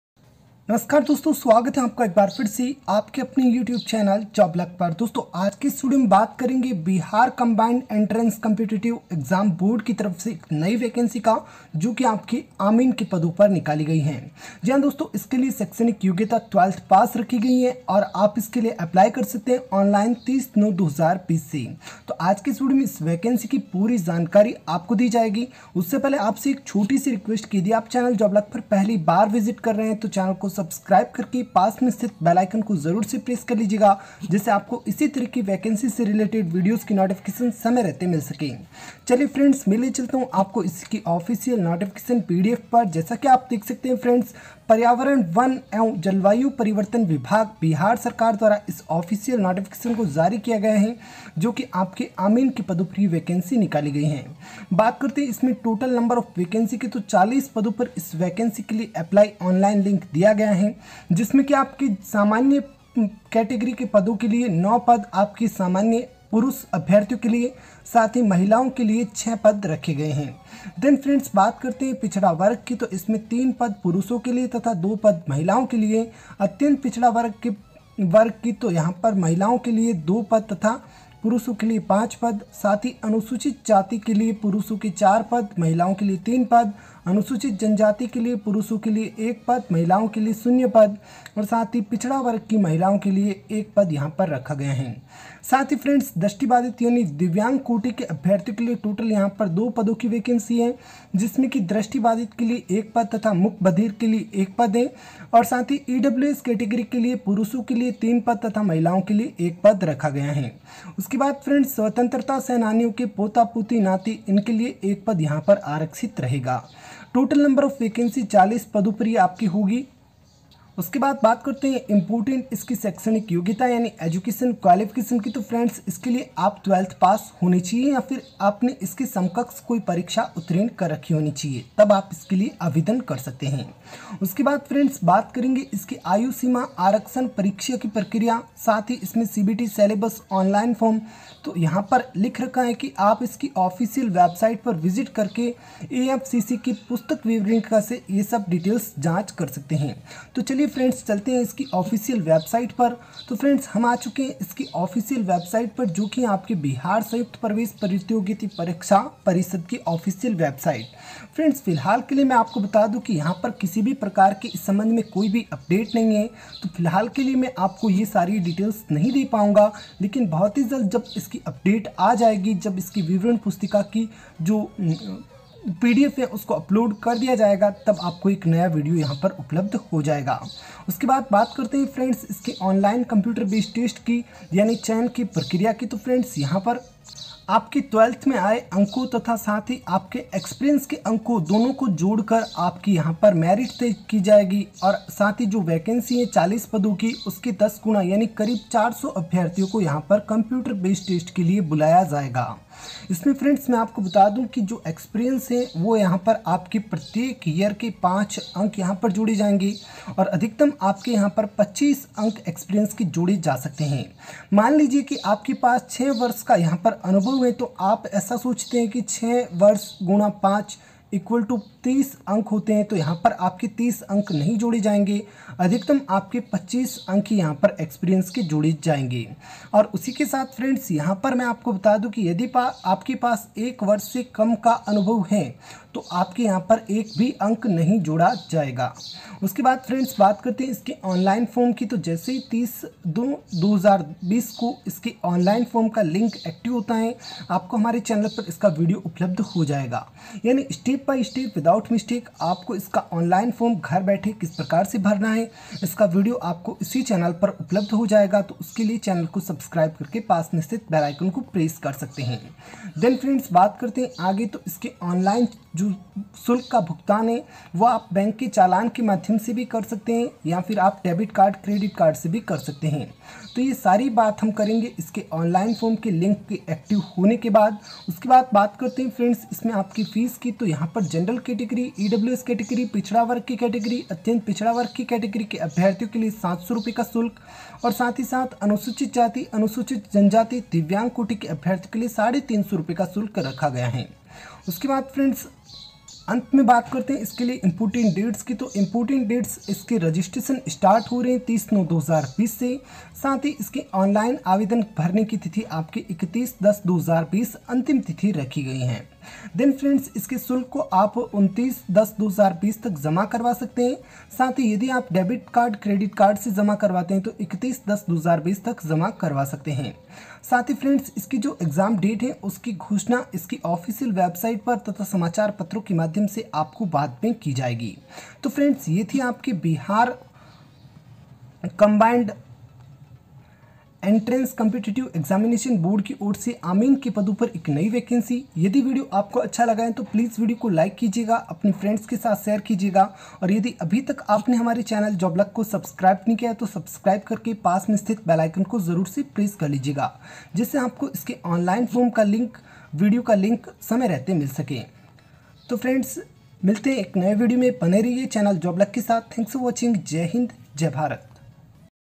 AMIN Recruitment 2020 in EFCC ll Bihar Combined Entrance Competitive Examination Board l Amin 2020
Bihar Combined Entrance Competitive Examination Board BCECEB Board and Department of Environment , Forest & Climate Change Are Invited to Online Application Form for the Recruitment Post of AMIN 2020. Those Candidates Are Interested & Eligible to Following Vacancies Can Read the Full Notification Before Apply Online.